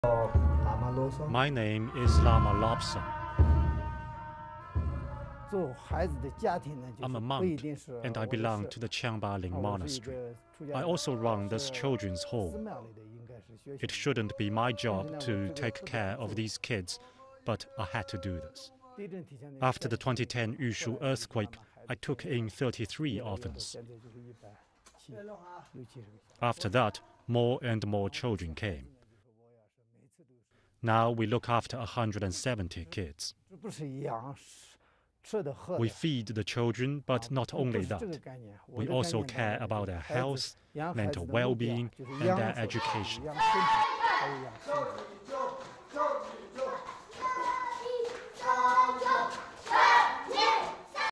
BBC News The Tibetan monk who has taken in 170 children
A Tibetan Buddhist monk took in 33 orphans after an earthquake in 2010 - now he is looking after 170 children.
Lama Lobsang originally started caring for the children on a temporary basis after the earthquake. "After that, more and more came," he says.
"It shouldn't be my job to take care of these kids but I had to do it," he says.
"They may be orphans, or from single-parent families," he says, "but I want to give them all a home."